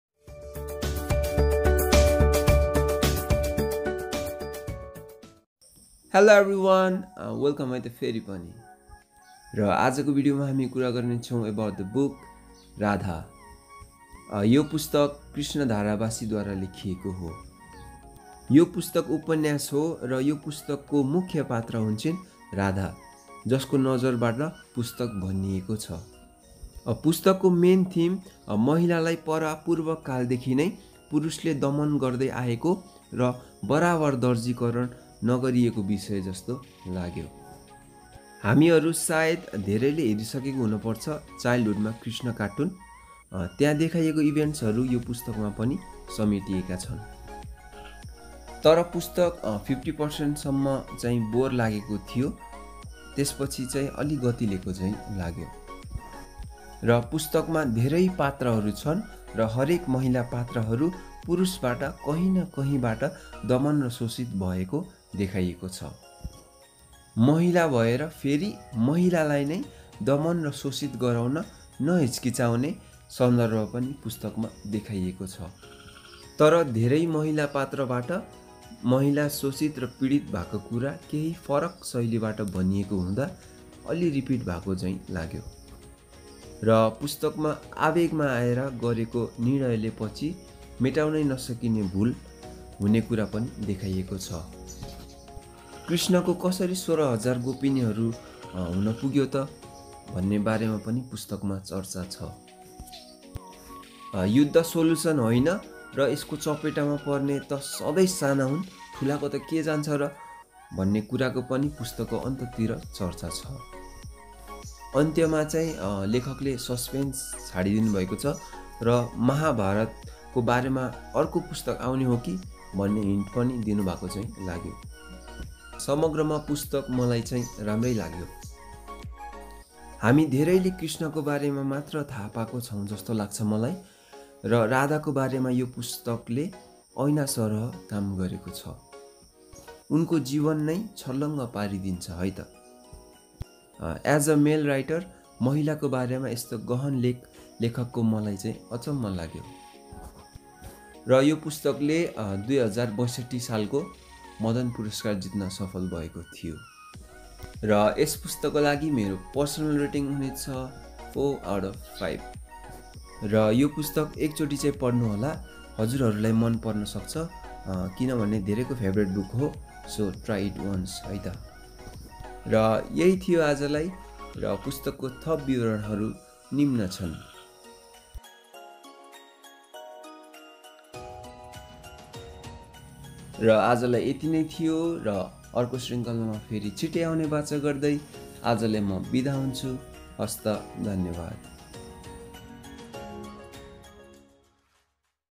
Radha | Krishna Dharabasi (Review)
A simple and brief review of the book "Radha".